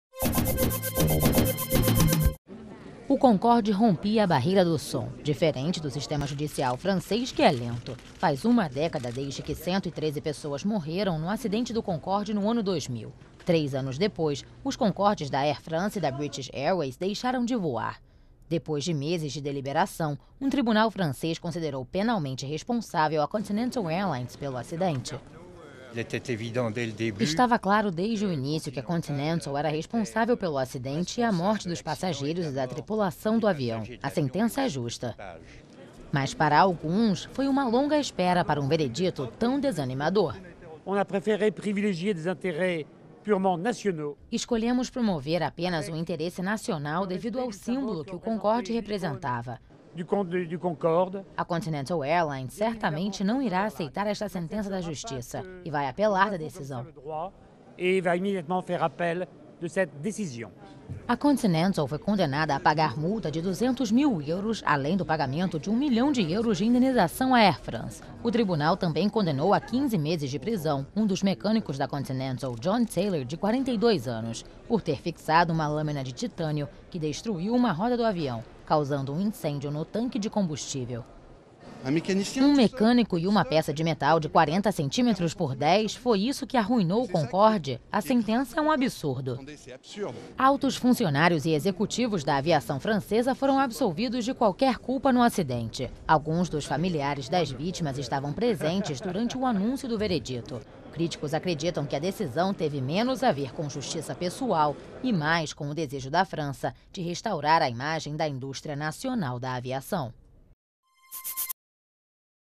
Concorde no tribunal
A justiça francesa considerou penalmente responsável nesta segunda-feira a Continental Airlines pelo acidente com o Concorde, que matou 113 pessoas no ano 2000, nas proximidades de Paris. A empresa também foi condenada a pagar multa de 200 mil euros além do pagamento de um milhão de euros de indenização à Air France.